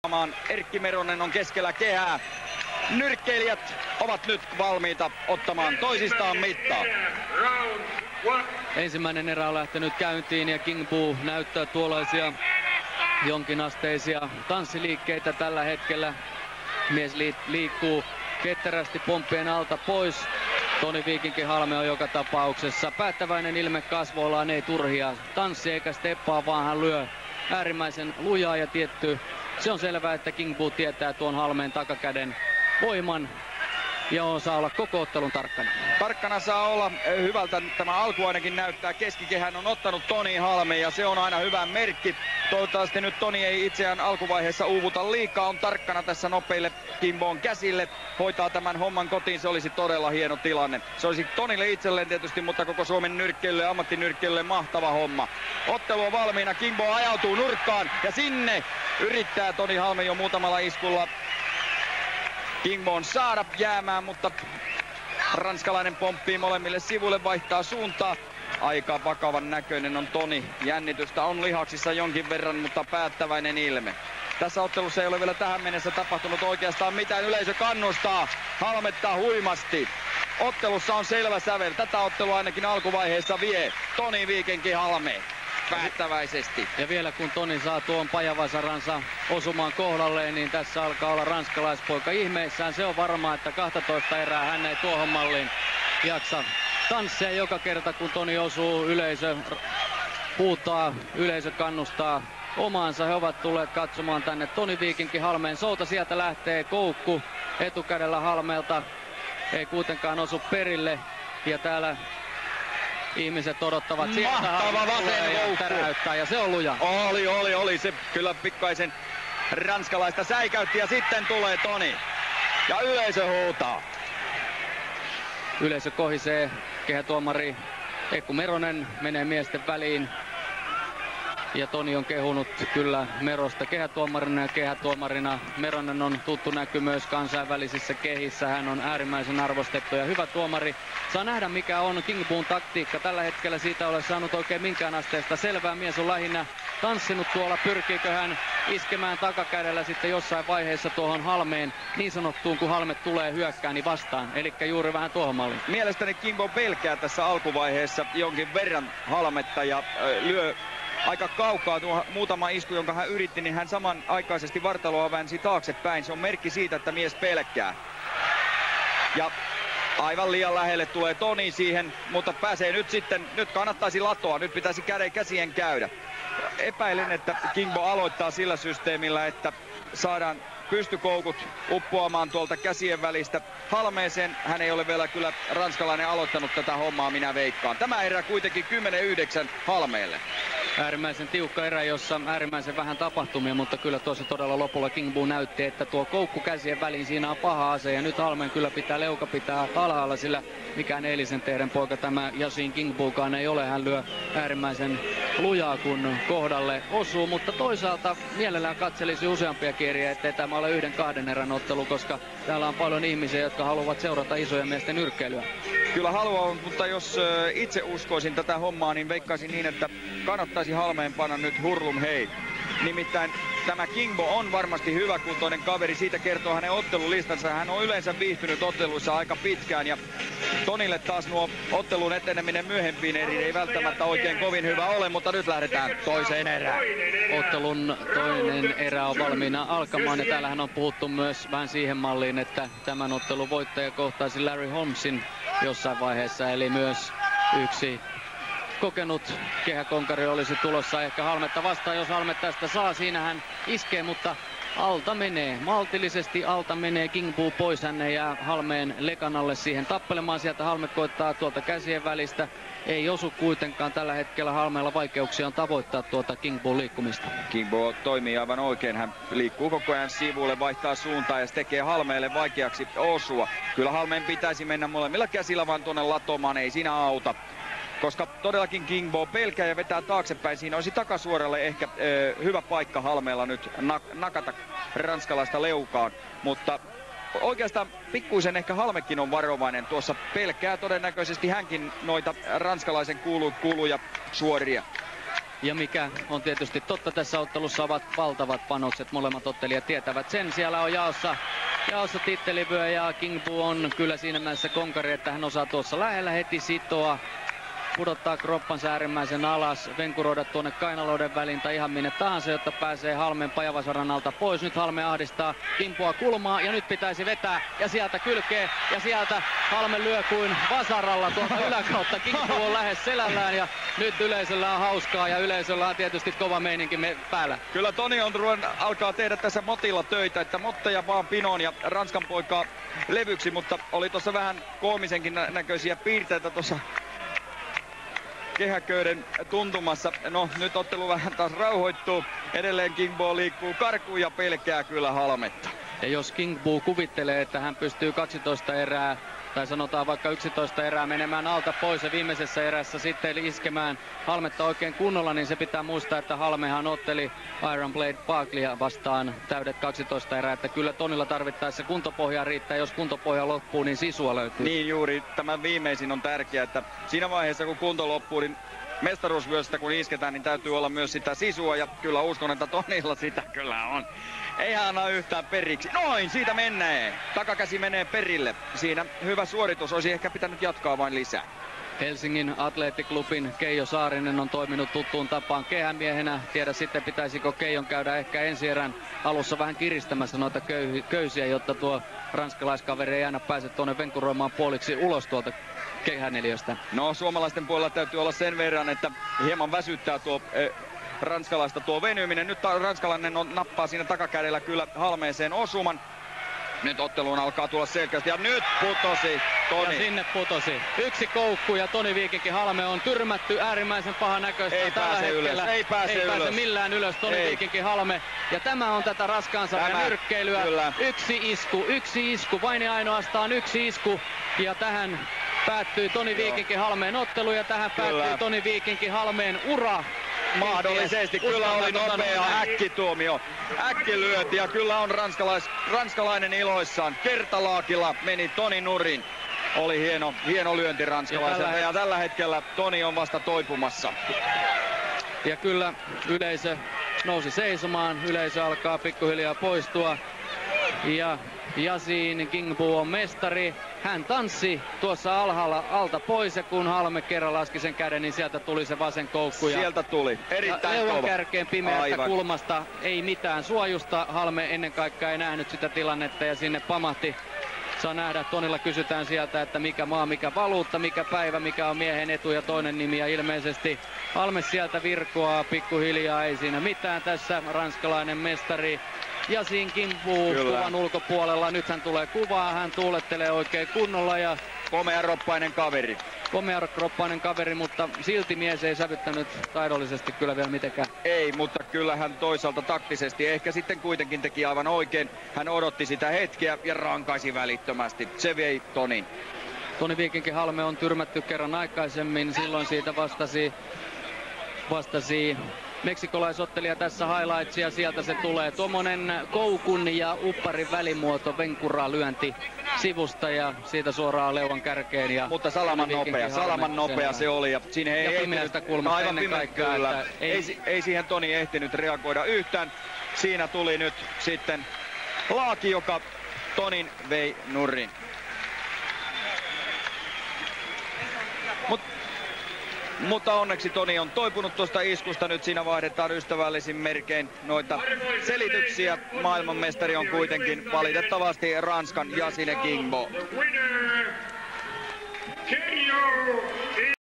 Tony Halme VS Yacine Kingbo NBA Heavyweight -titteliottelu
3. lokakuuta 2000 Helsinki